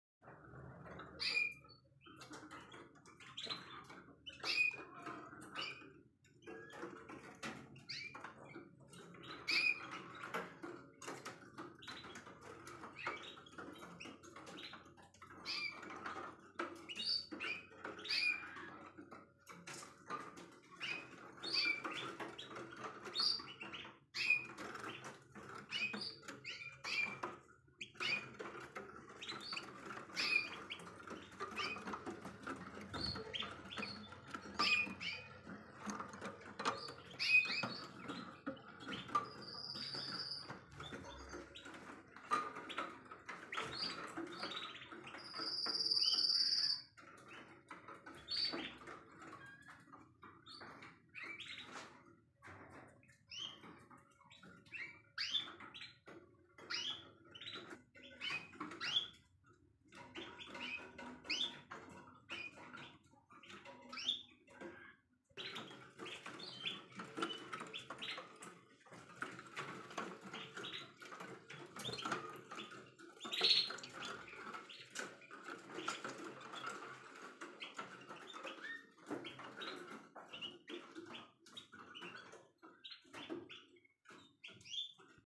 Vše v plném proudu 🥚🥚🥚 sezóna 2023 samičky snášejí
Samičky stavějí hnízda a zítra mi zasedne další již 2 samička. A další budou přibývat. Jen ať jsou oplozená 👍🥚🐦